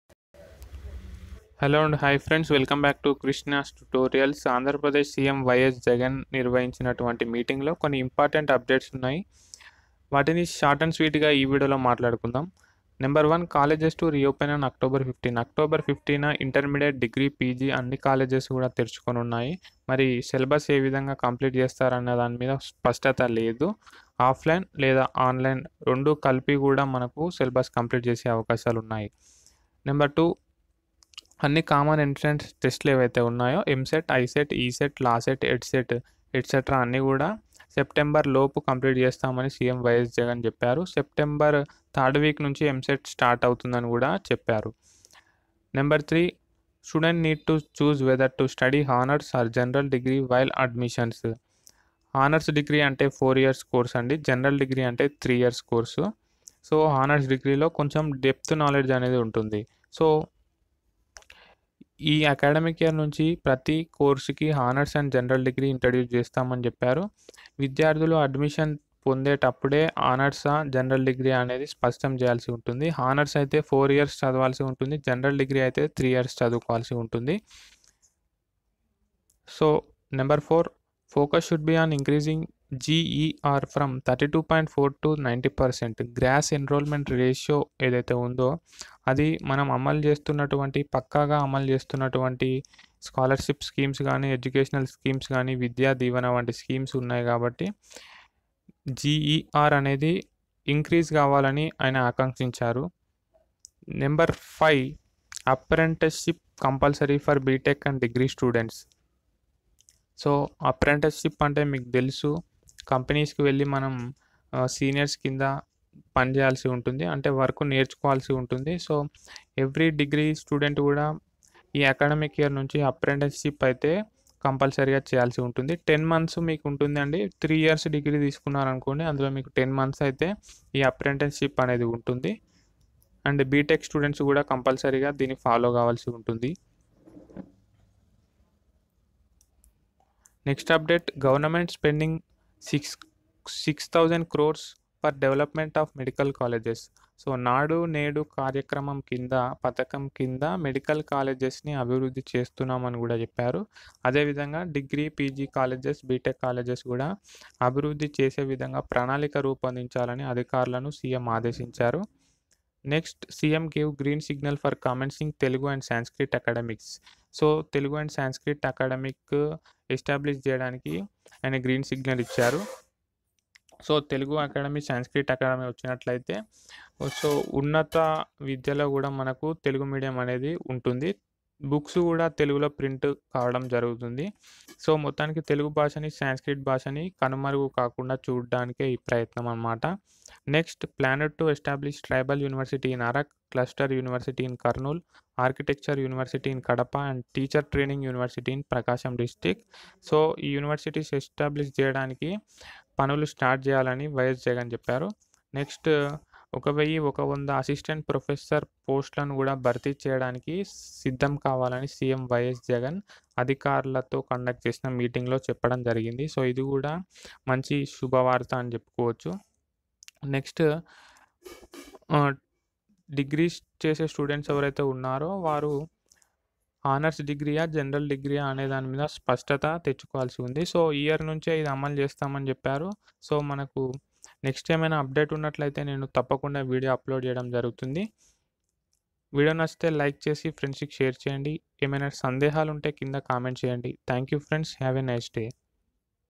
COLLEGES IN AP TO OPEN ON OCTOBER 15 || NEWS ON HONORS DEGREE| CM YS JAGAN MEETING LATEST UPDATES
WELCOME TO KRISHNA'S TUTORIALS. !,!😊

Also, visit LL.B. playlist for more informative videos.